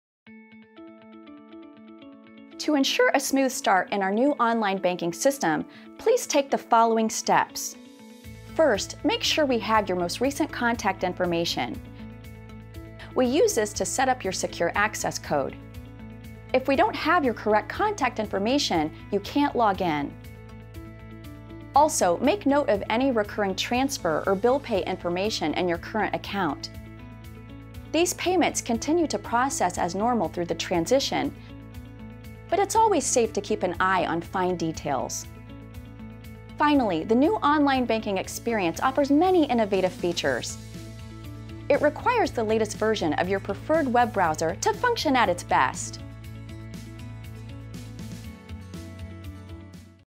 Preparing for Digital Banking
Get a preview of what you can expect with our new Digital Banking!